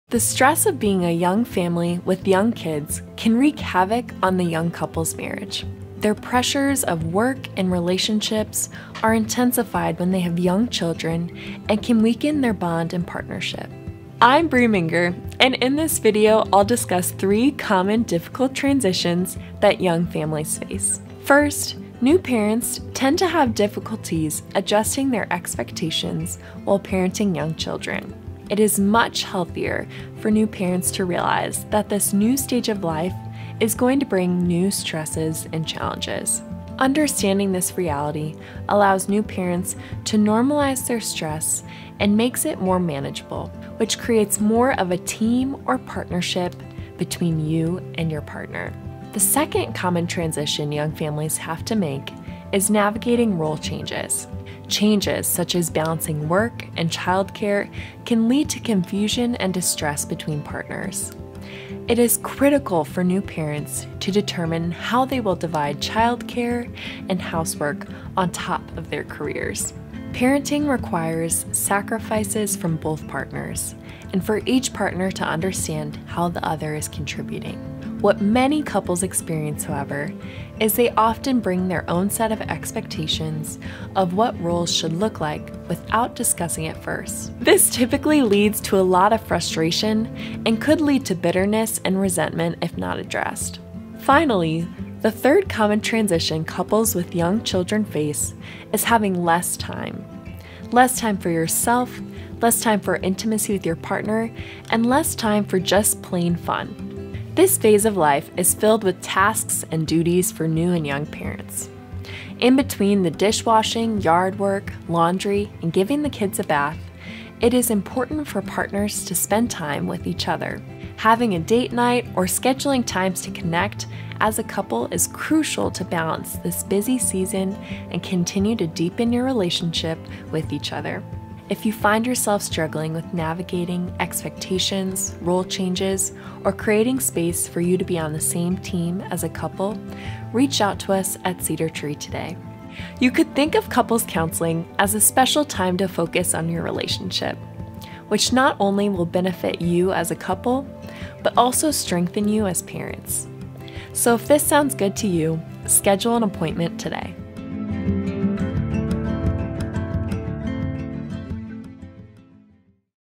3 Difficult Transitions Young Families Face
The stress of being a young family with young kids can wreak havoc on the young couple’s marriage. Their pressures of work and relationships are intensified when they have young children and can weaken their bond and partnership.

In this video, Bree Minger, one of our newest clinicians, discusses three common difficult transitions that young families face.